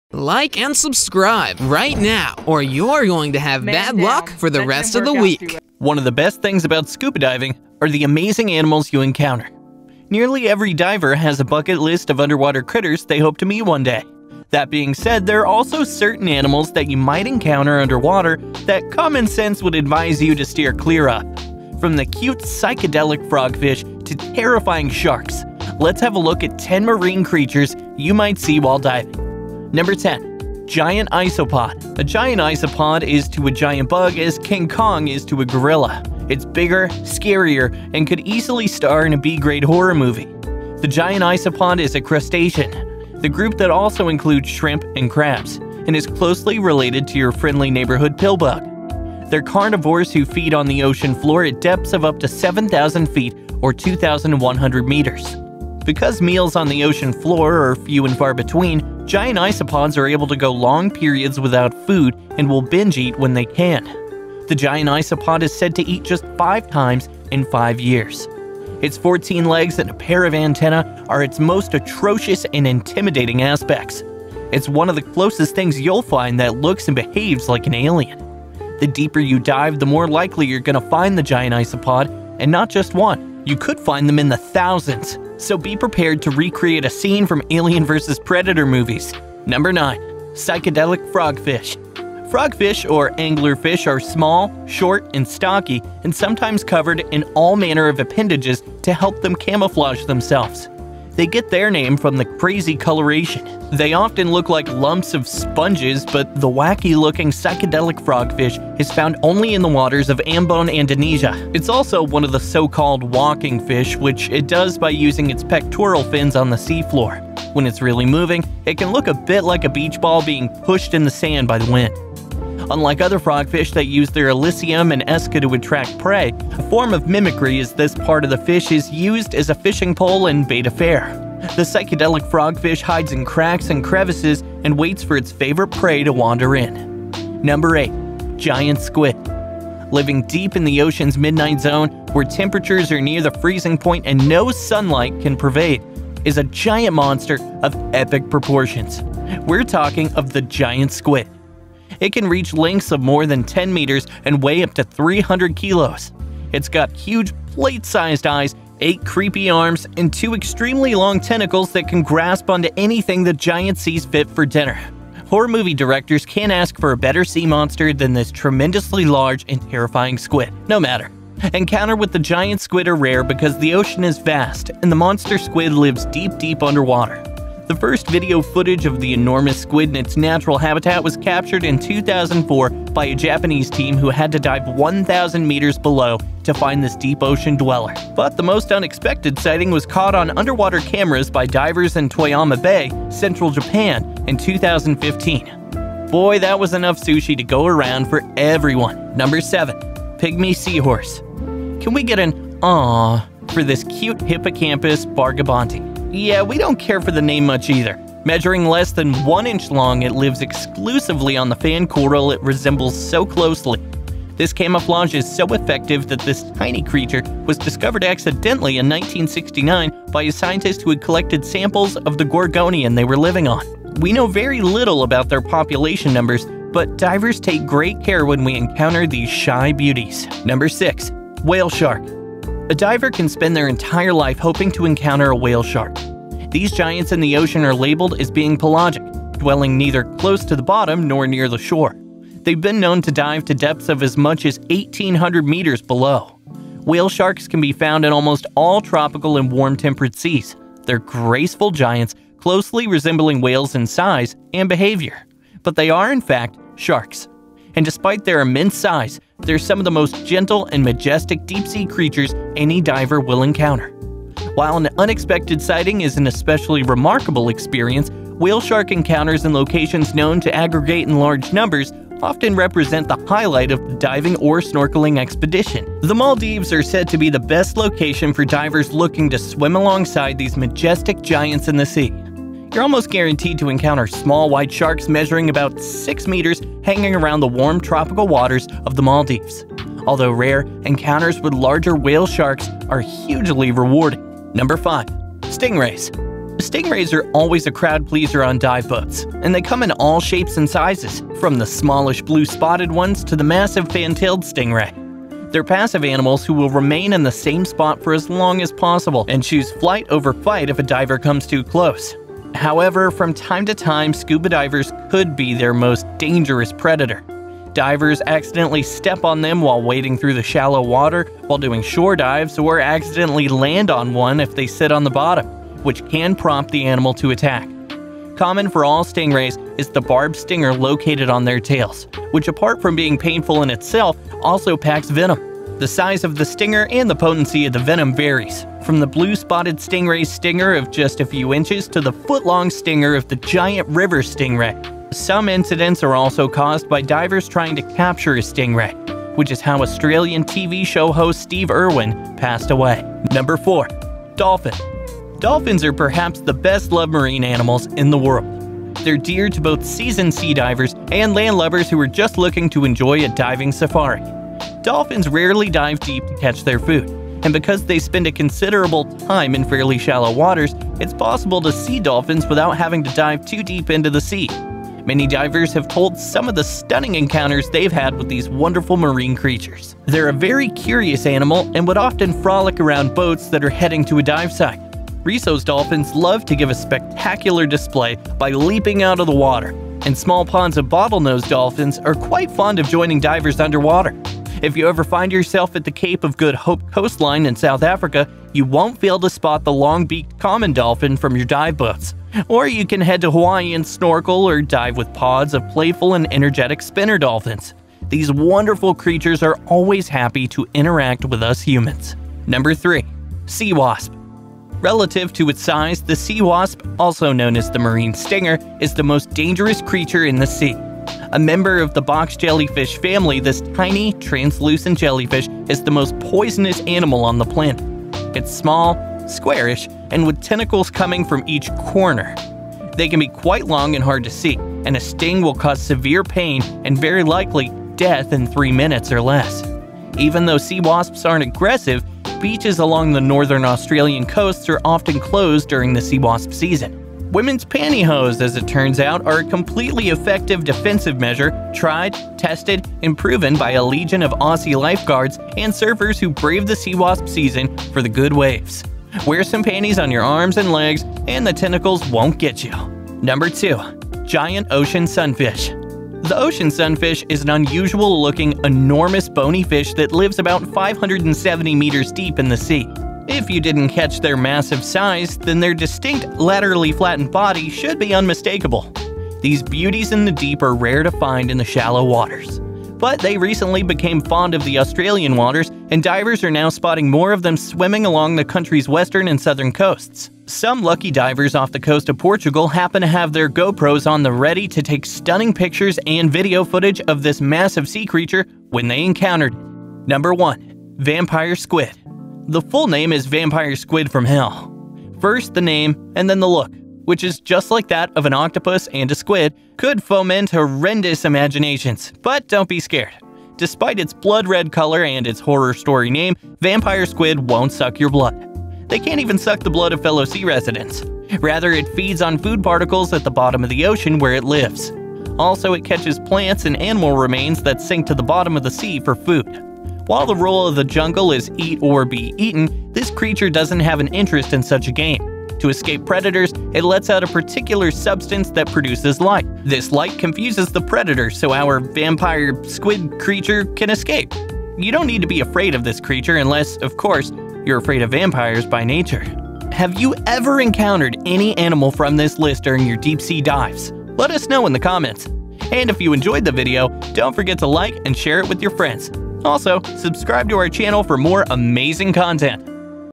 10 Deep Sea Creatures Faced By Divers
10 Deep Sea Creatures Faced By Divers

One of the best things about scuba diving is the amazing animals you encounter. Nearly every diver has a bucket list of underwater critters they hope to meet one day. That being said, there are also certain animals that you might encounter underwater that good sense would advise you to steer clear of. From the cute Psychedelic Frogfish to terrifying sharks, let's have a look at ten marine creatures you might see while diving!
Number 10. Giant Isopod
A giant isopod is to a giant bug as King Kong is to a gorilla: it's bigger, scarier, and could easily star in a B-grade horror movie. The giant isopod is a crustacean, the group that also includes shrimp and crabs, and is closely related to your friendly neighborhood pillbug. They are carnivores who feed on the ocean floor at depths up to 7,000 feet (2,100 meters). Because meals on the deep ocean floor can be few and far between, giant isopods are able to go long periods without food and will binge eat when they can. The giant isopod is said to eat just five times in five years.
Its 14 legs and a pair of antennas are its most atrocious and intimidating aspects. It is one of the closest things you will find that looks and behaves like an alien. The deeper you dive, the more likely you're going to be to find the giant isopod and not just one; they could be in quantity of thousands. So be prepared to recreate a scene from Aliens Vs. Predator movies!
Number 9. Psychedelic Frogfish
Frogfish or anglerfish are small, short and stocky, and sometimes covered in all manner of appendages to help them camouflage themselves. They get their name for their crazy coloration. They often can look like lumps of sponges. But the wacky-looking psychedelic frogfish is found only in the waters around Ambon, Indonesia. It's also one of the so-called walking fish, which it does by using its pectoral fins on the seafloor — when it's really moving, it can look a bit like a beach ball being pushed on the sand by wind. Unlike other frogfish that use their illicium and esca to attract prey — a form of mimicry as this part of the fish is used as a fishing-pole-and-bait affair — the psychedelic frogfish hides in cracks and crevices and waits for its favorite prey to wander in.
Number 8. Giant Squid
Living deep in the ocean's midnight zone, where temperatures are near the freezing point and no sunlight can pervade, is a giant monster of epic proportions. We're talking of the giant squid. It can reach lengths of more than 10 meters and weigh up to 300 kilos. It's got huge plate-sized eyes, eight creepy arms, and two extremely long tentacles that can grasp onto anything the giant sees fit for dinner. Horror movie directors can't ask for a better sea monster than this tremendously large and terrifying squid.
No matter encounters with the giant squid are rare because the ocean is vast, and the monster squid lives deep underwater. The first video footage of the enormous squid in its natural habitat was captured in 2004 by a Japanese team who had to dive 1,000 meters below to find this deep-ocean dweller. But the most unexpected sighting was caught on underwater cameras by divers in Toyama Bay, central Japan, in the charismas of 2015. Boy, that was enough sushi to go around for everyone.
Number 7. Pygmy Seahorse
Can we get an awwww for this cute Hippocampus bargibanti? Yeah, we don't care for the name much either. Measuring less than 1 inch long, it lives exclusively on the fan coral it resembles so closely. This camouflage is so effective that this tiny creature was discovered accidentally in 1969 by a scientist who had collected samples of the gorgonian they were living on. We know very little about their population numbers, but we divers take great care when we encounter these shy beauties.

Number 6. Whale Shark
A diver can spend their entire life hoping to encounter a whale shark. These giants in the ocean are labeled as being pelagic – dwelling neither close to the bottom nor near the shore. They have been known to dive to depths of as much as 1,800 meters below. Whale sharks can be found in almost all tropical and warm-temperate seas. They are graceful giants, closely resembling whales in size and behavior, but they are, in fact, sharks. And despite their immense size, they are some of the most gentle and majestic deep-sea
creatures any diver will encounter.